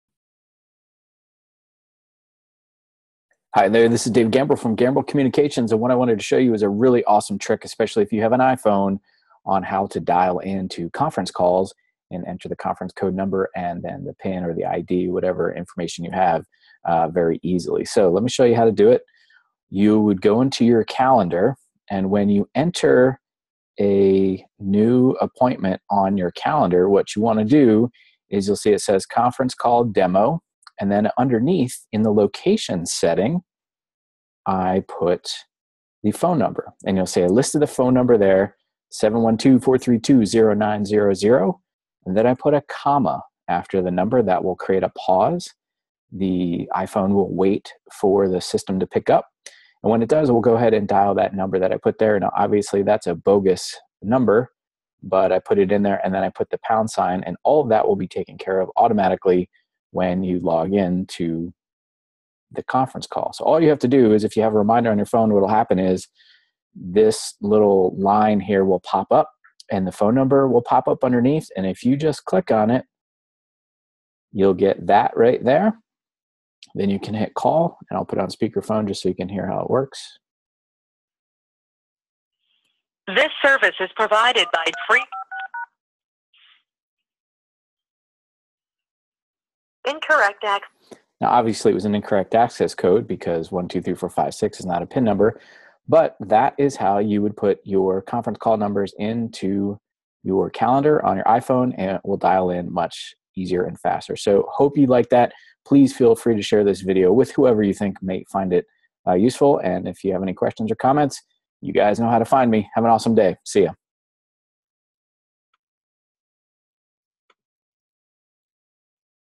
Conference Call Shortcut for iPhones
This 2 minute video shows you how to login to conference calls much easier and faster on an iPhone